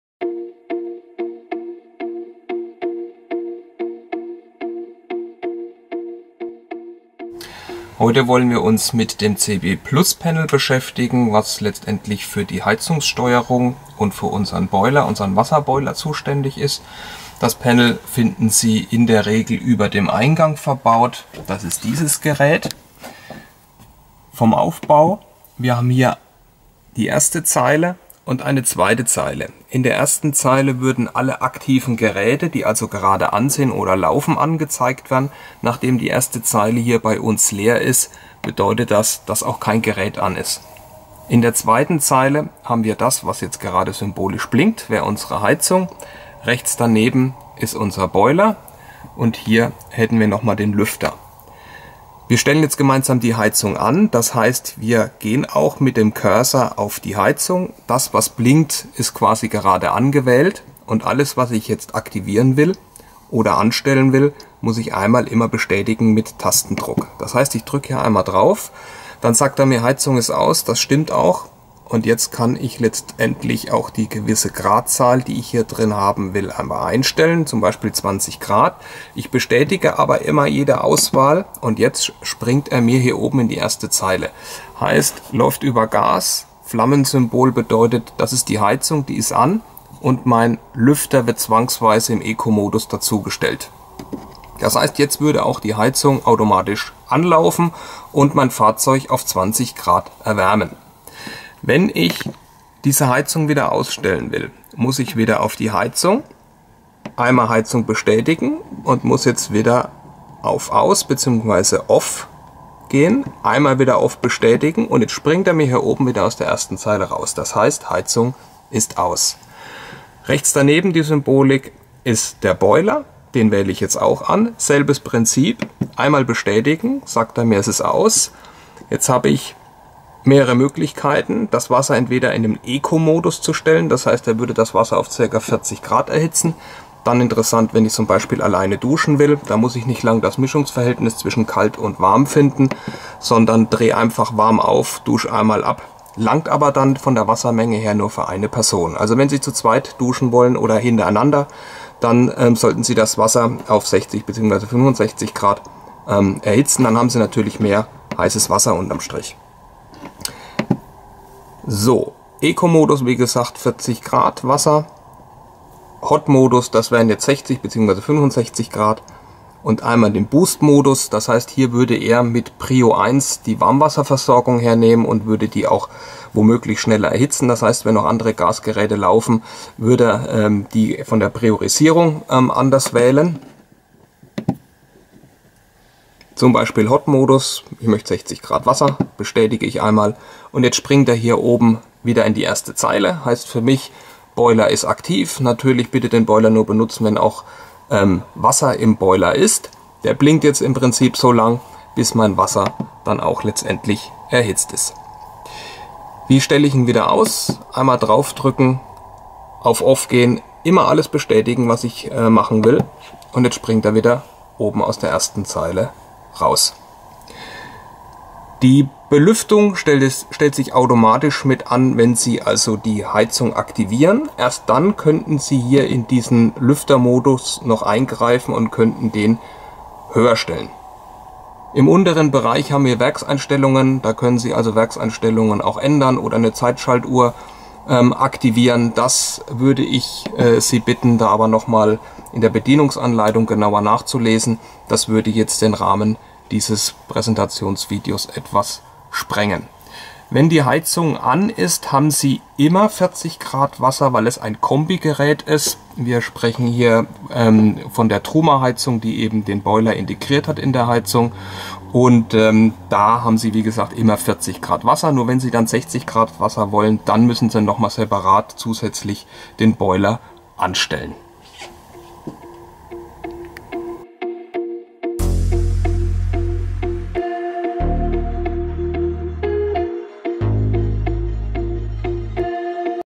Truma Control CP Plus Panel - Unsere Anleitung
In diesem Video beschäftigen wir uns mit dem Truma CP Plus Panel, dass für die Heizungssteuerung und den Wasserboiler in deinem Camper Van oder Reisemobil zuständig ist.
Schauen Sie sich unsere Anleitung an, um alle Funktionen zu verstehen und zu nutzen.

00:00 - Intro
00:07 - Heizung einschalten und konfigurieren
01:42 - Heizung ausschalten
02:00 - Boiler einschalten und konfigurieren
03:26 - Boiler ausschalten
03:36 - Belüftung
03:51 - Werkseinstellungen